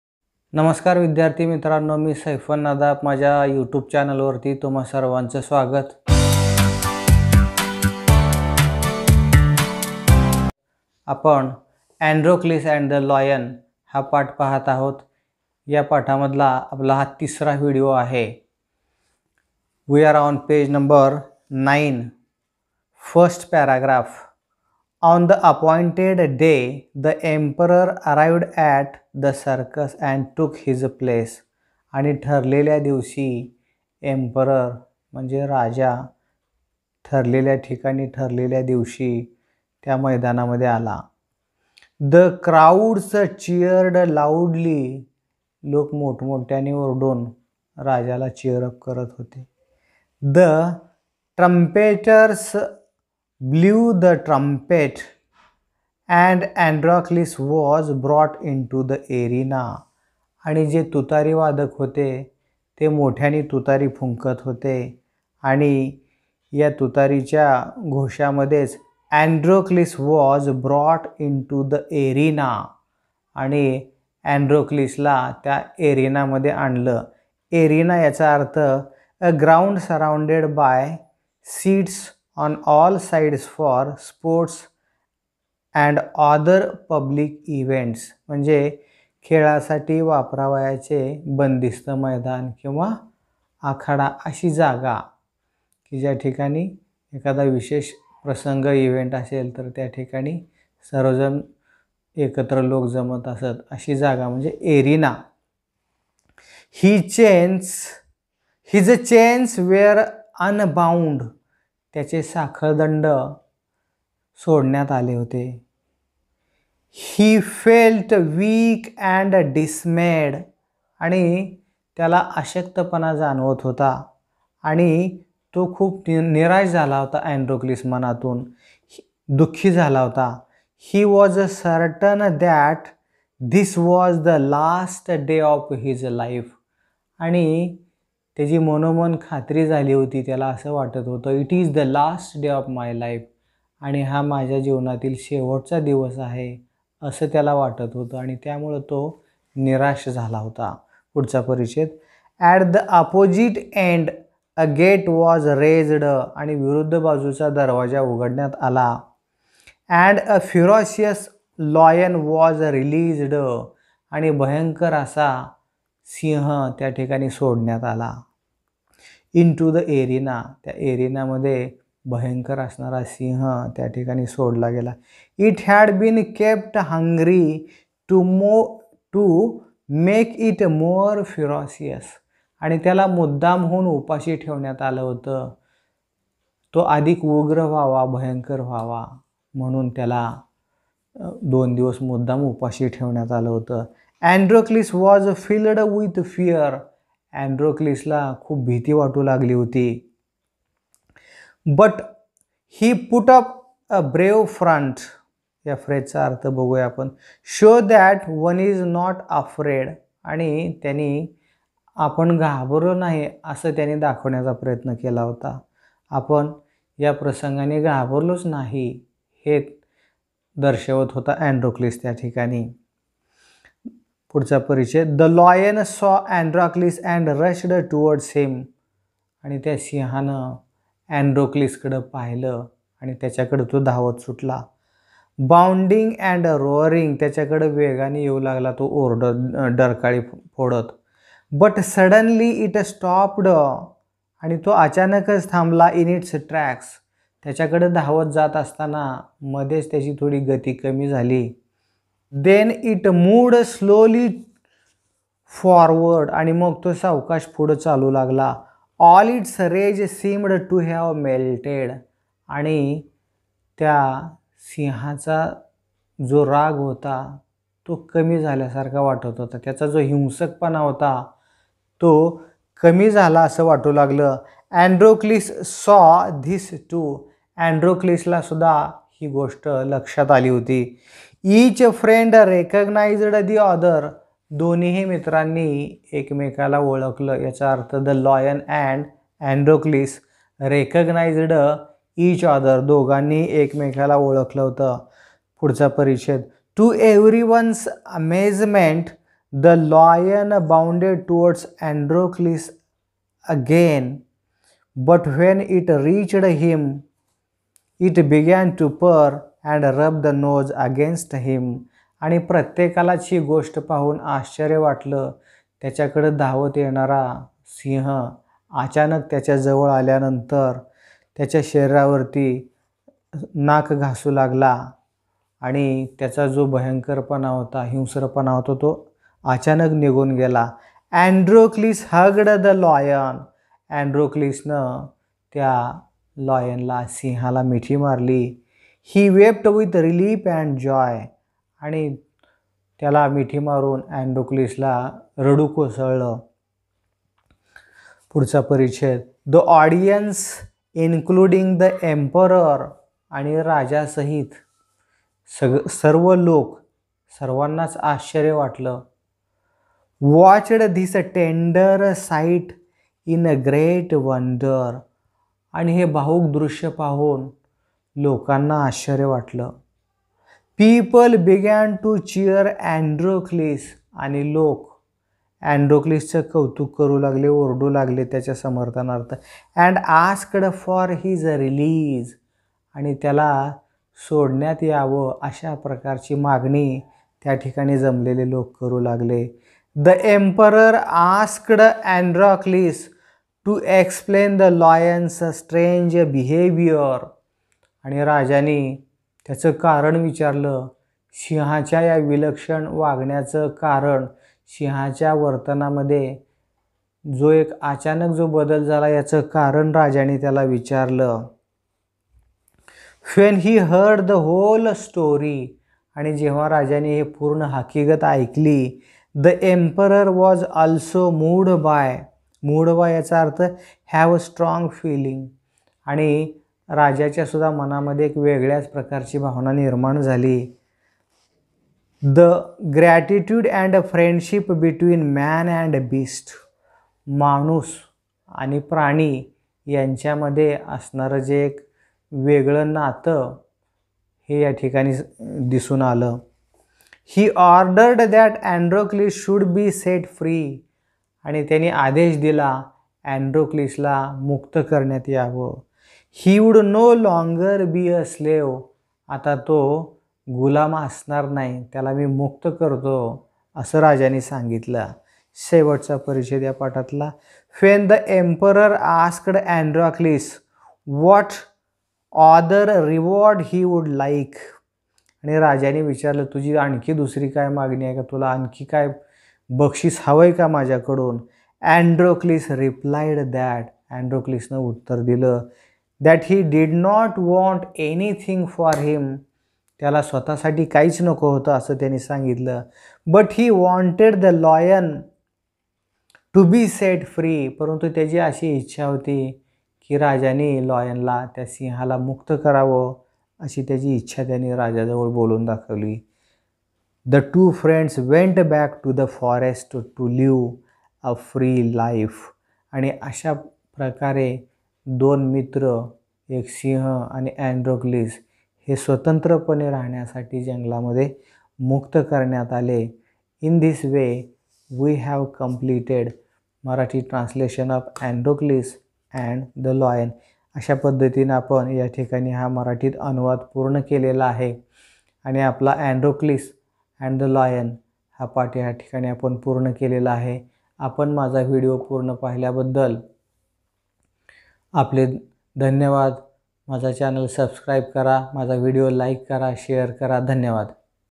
1.2 Androcles and the Lion | std 8th | explanation in Marathi | By Saifan Nadaf |part 3
Androcles and the Lion
English lesson
Std 8th
Saifan Nadaf
Maharashtra board
Marathi medium